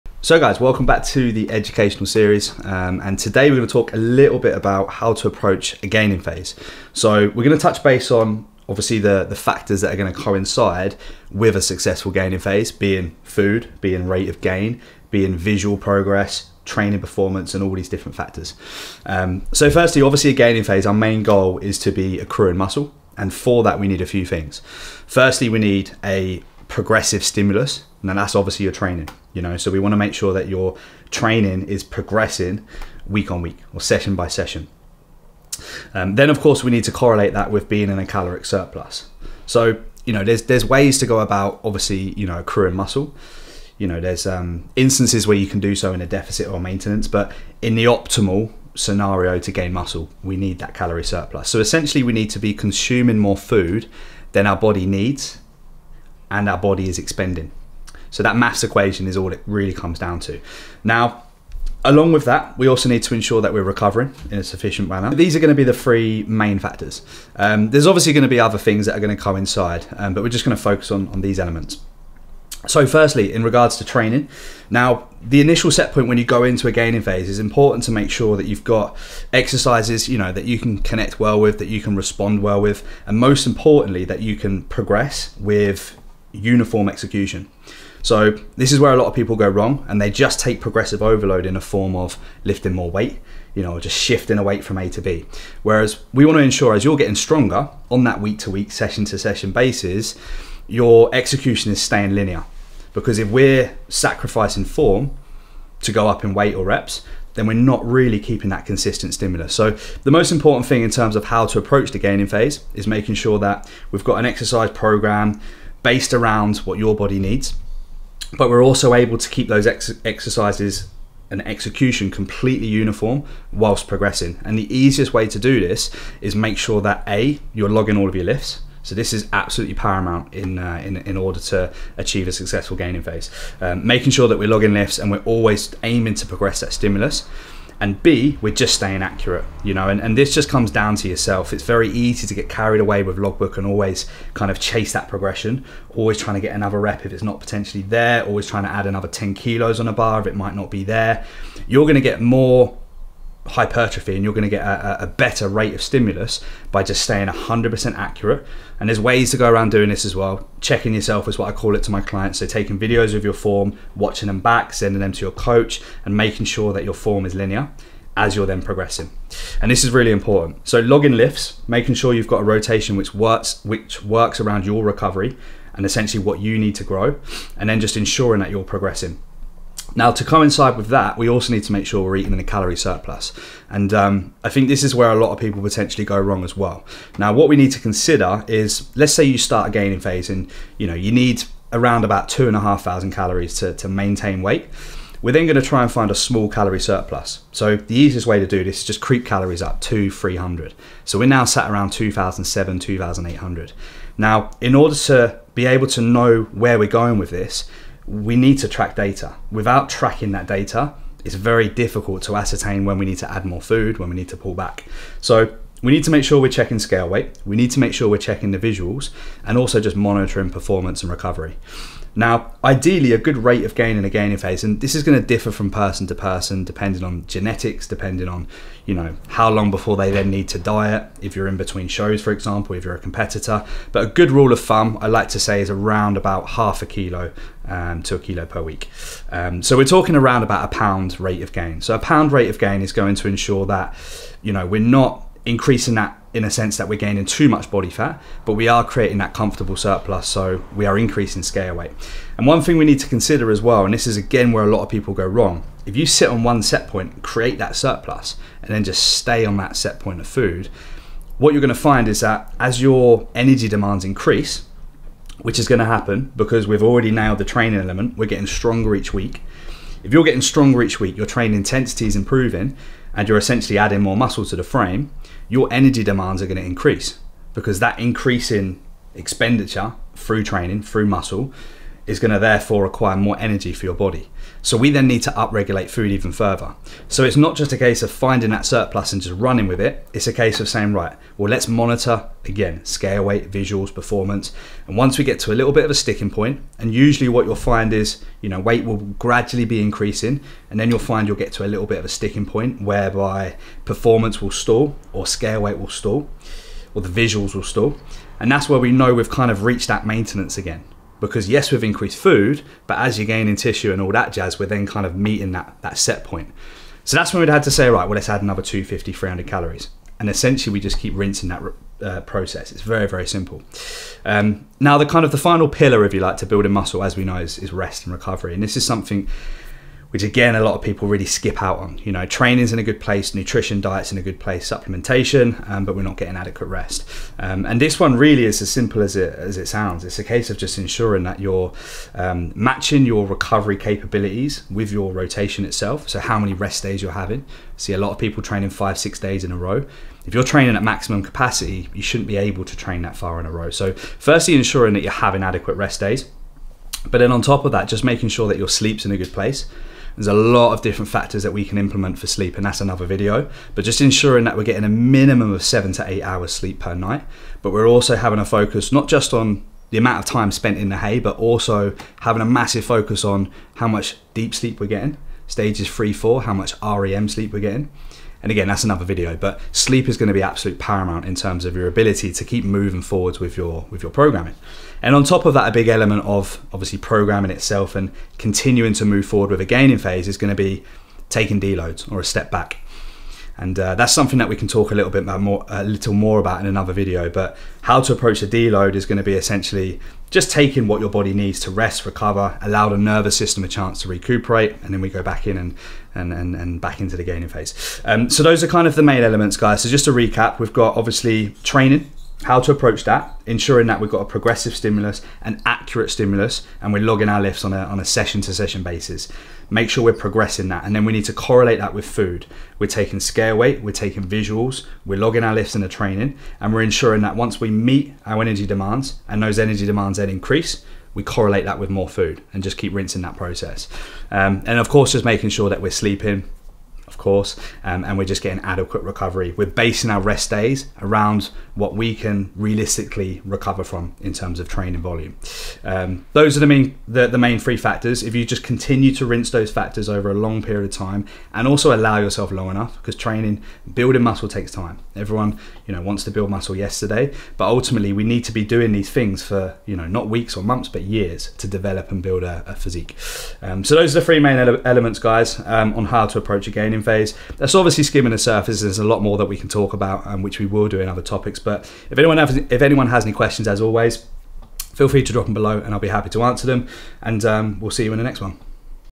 How To Approach A Gaining Phase / The Educational Series
In this video i discuss how to approach your gaining phase when looking to build muscle, what we should look for in terms of rate of gain and how to structure training, nutrition and recovery.